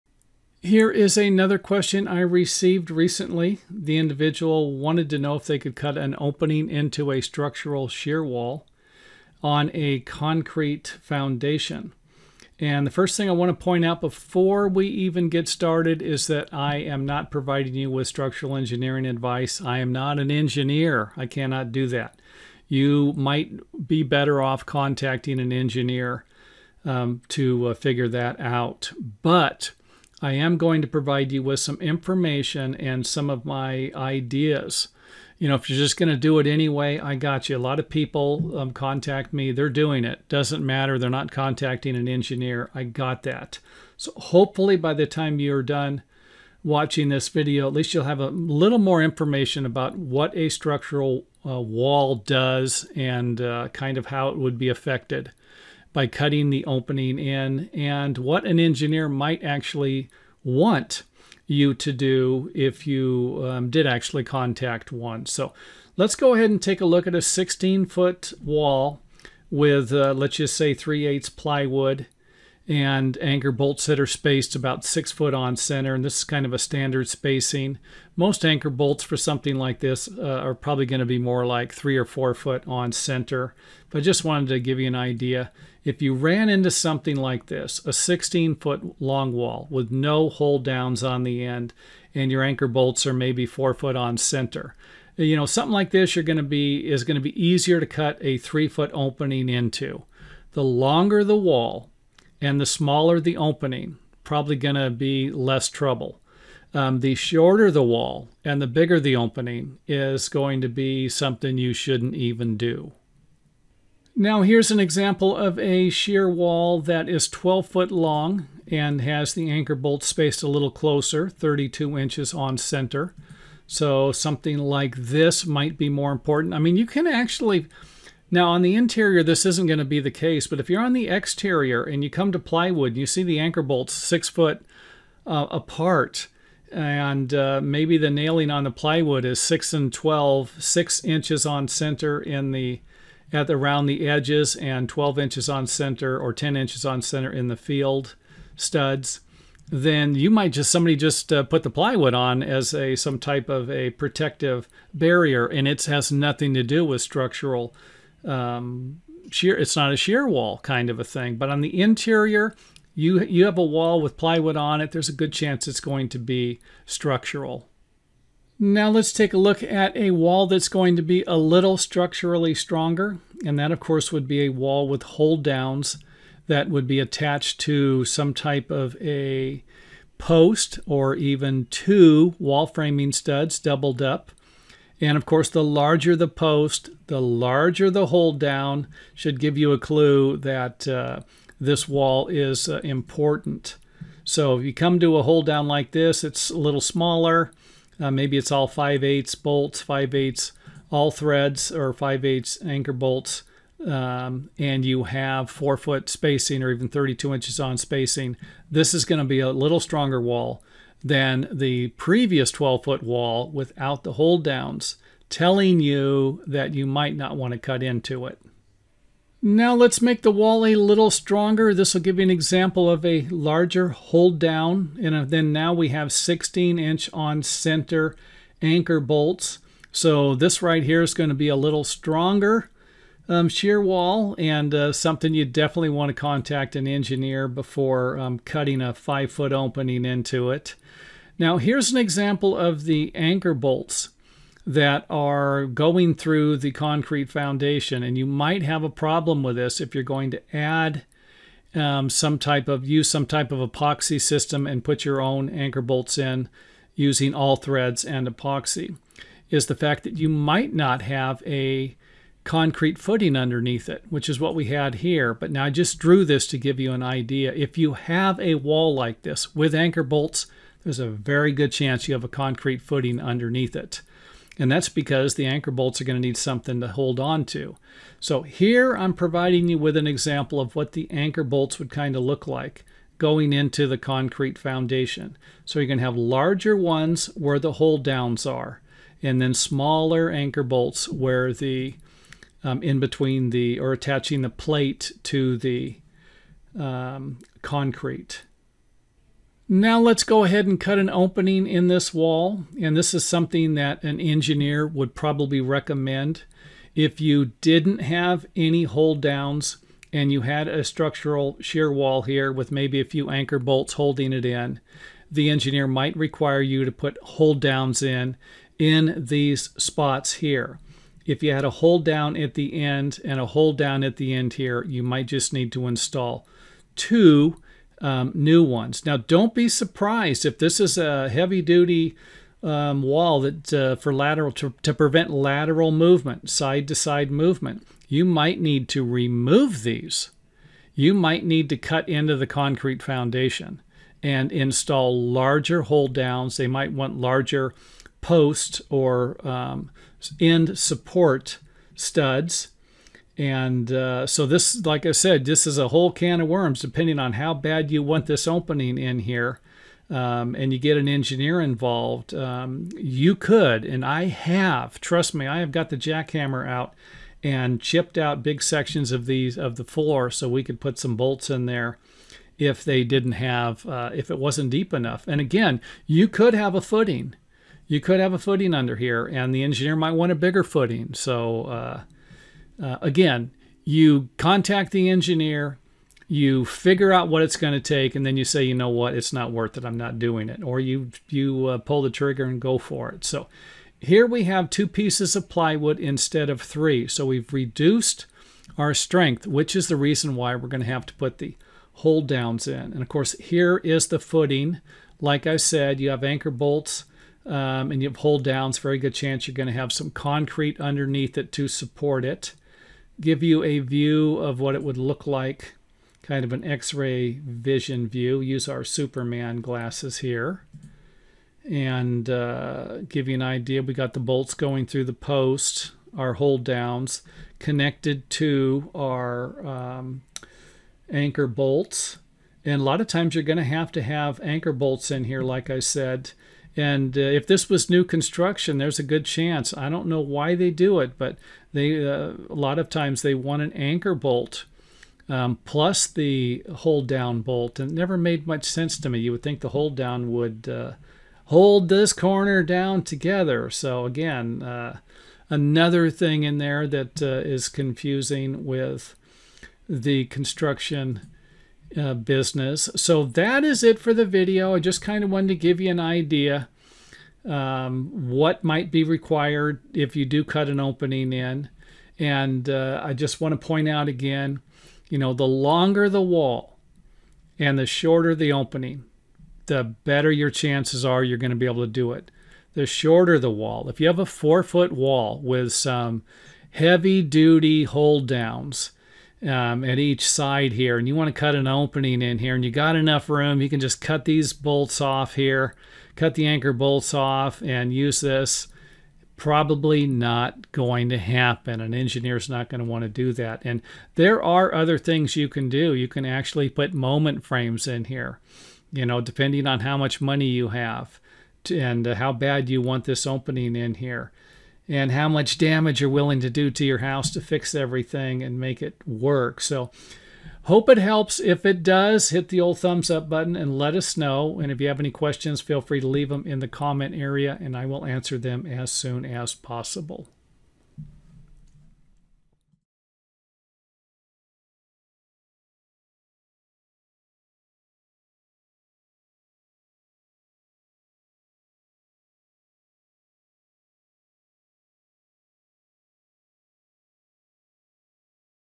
Watch This Video Before Cutting Opening In Structural Shear Wall Framing
Click on this link for more helpful videos about wall framing, home remodeling and building repairs. In this video I will provide you with some extremely helpful information that you as a do-it-yourselfer should be familiar with, before you decide to cut an opening into a structural shear wall that was put there to reduce lateral movement by an engineer. Smaller walls will be more difficult and longer ones might be a little easier to cut openings into.